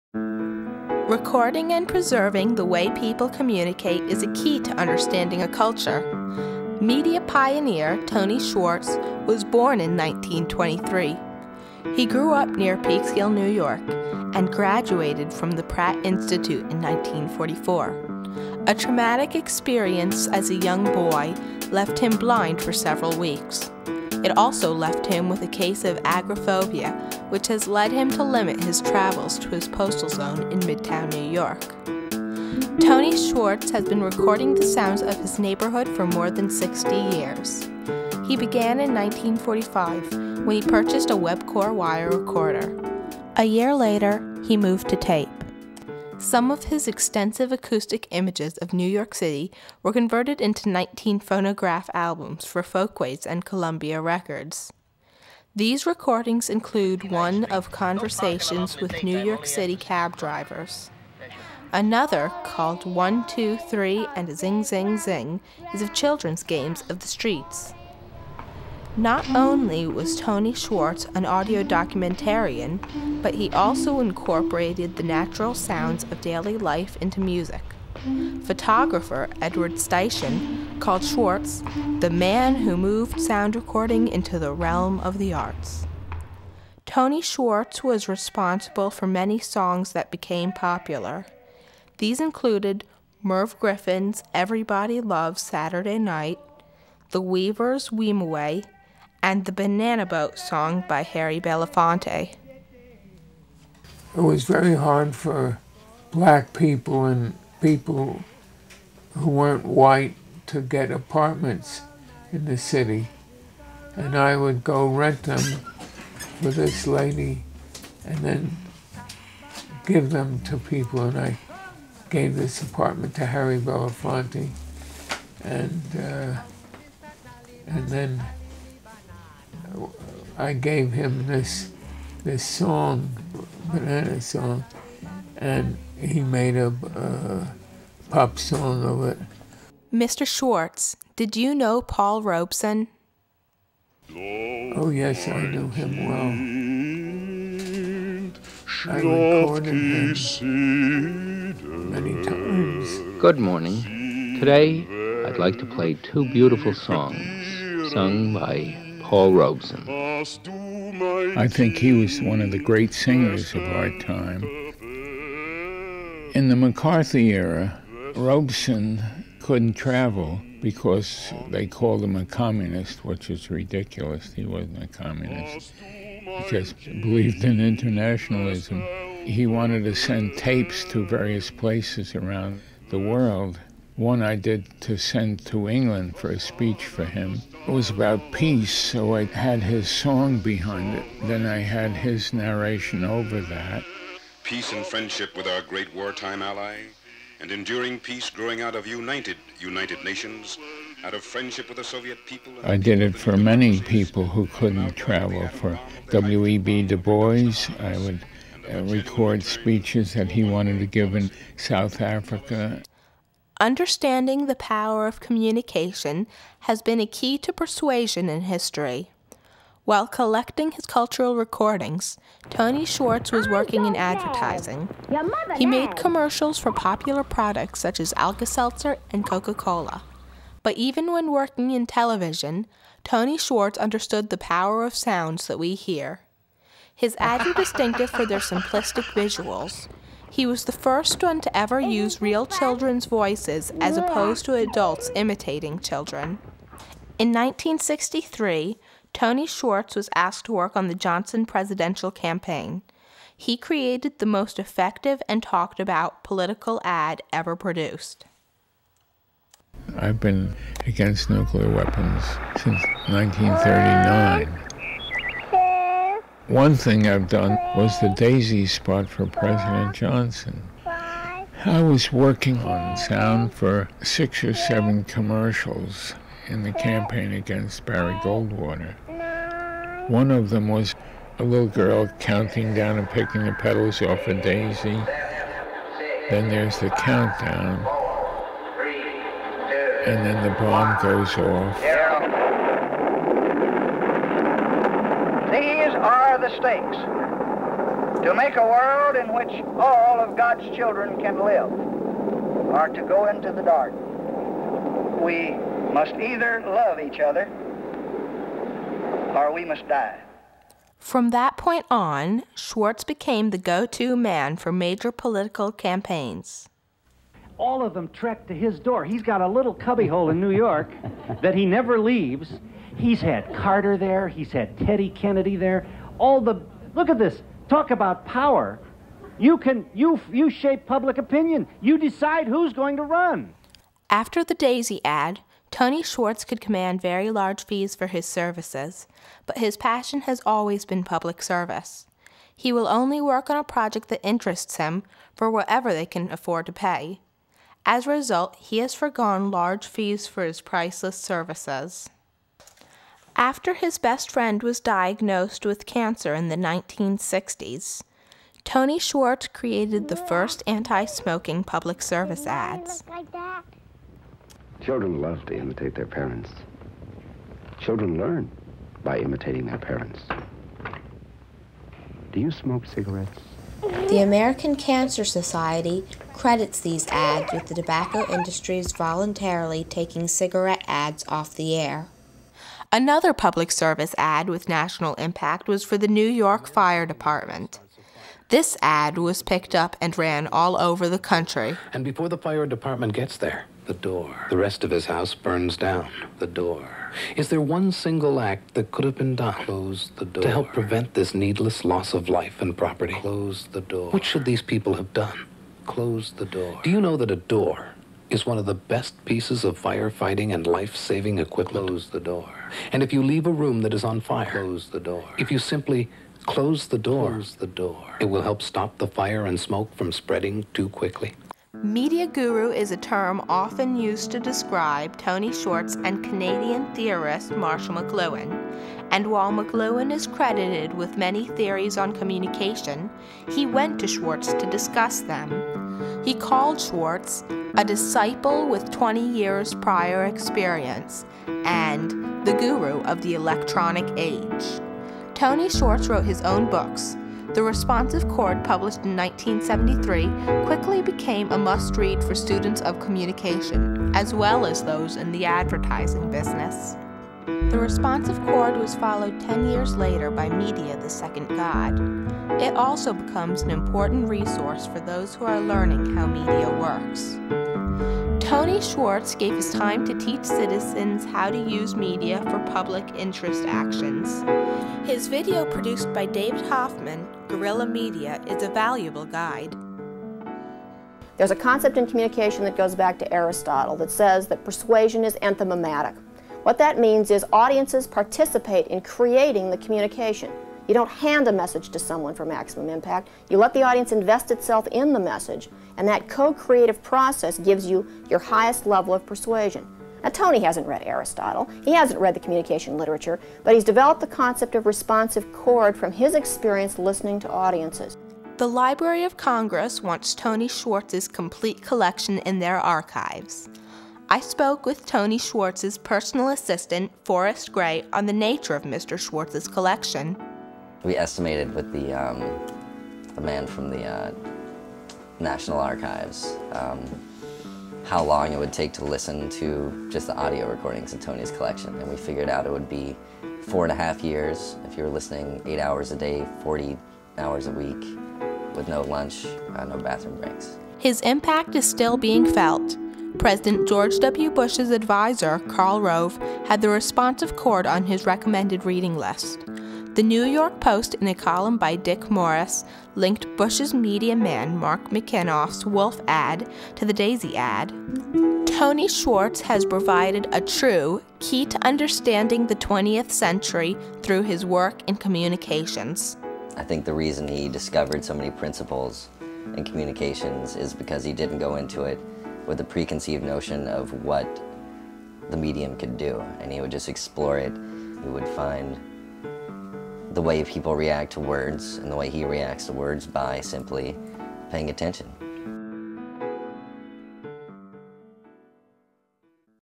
Tony Schwartz: Communications Archivist, Theorist, Innovator
Documentary about Tony Schwartz for the National History Day Competition by Emily Hughston Hoffman

This is the version I made for the high school level in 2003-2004. This video went through several incarnations before it reached it's final version which made it to the Run-offs on the National level. As soon as I am able to track down the quicktime file of my final version, I'll upload that.